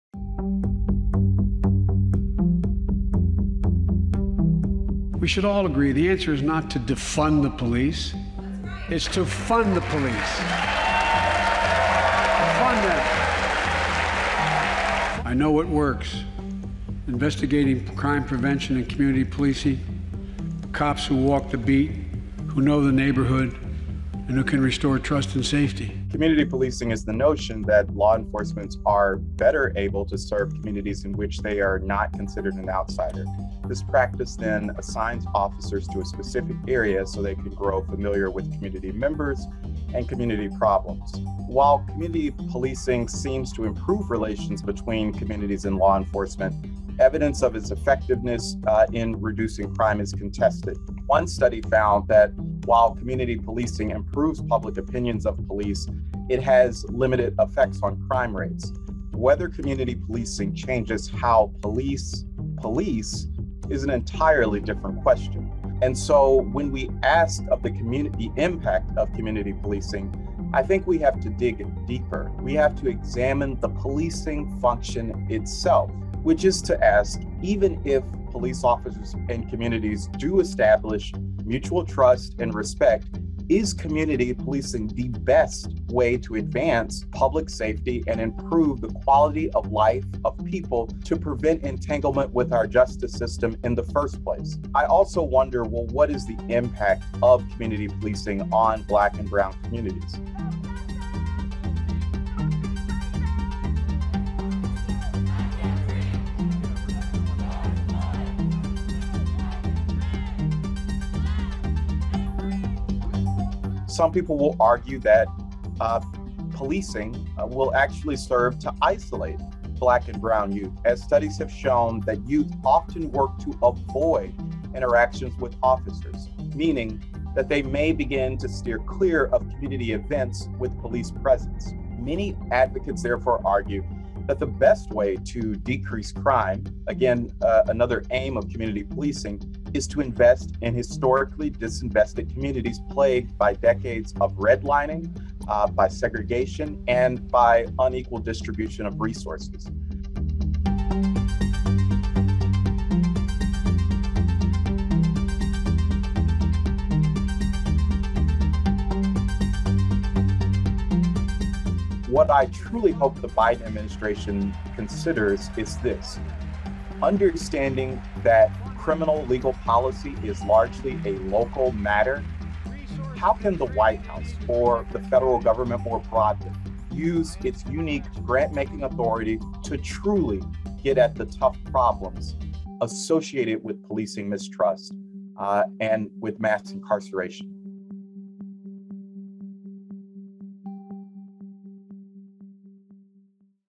The Reality of Community Policing
This criminal justice expert & attorney says community policing doesn't work — here's why